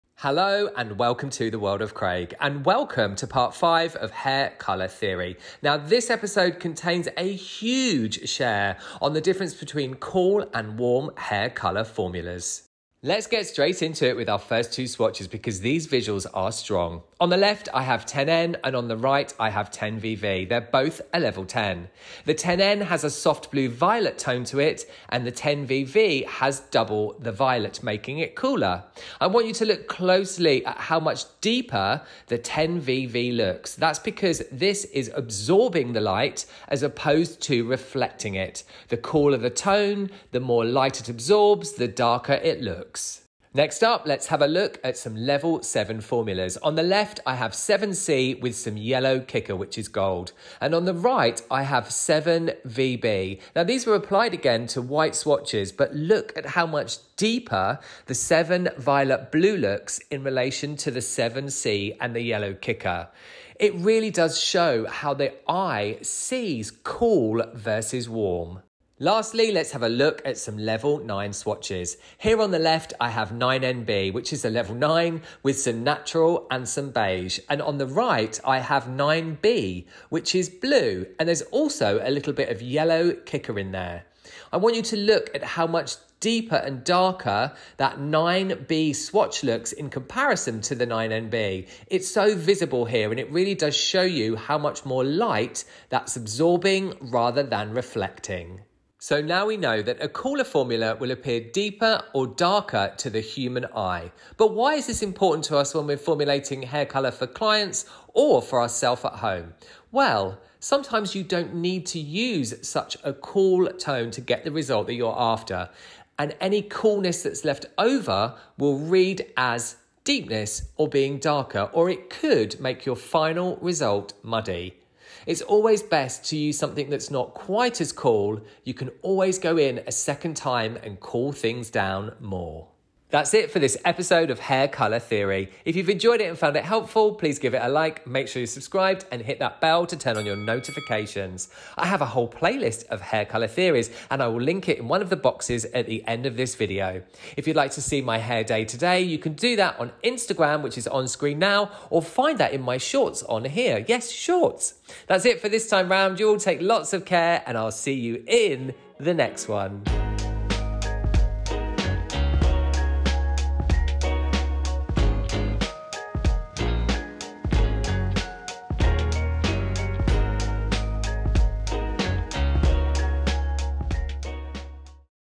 Hair Color Theory Ep 5 - COOL vs WARM...this is FUNDAMENTAL. Featuring Shades EQ Gloss.
Hi there
Happy New Year! I thought we'd start the NEWNESS with a BANG and another episode of my hair color theory series...this is a BIGGIE!
Understanding the difference a cool/warm formula can have on a result plays a huge part in the result...

It would be fantastic to hear from you in the comments - did you find this video helpful? Remember, I reply to every, single, comment. It's always great to be in touch...
Till the next one,

                                                            CX.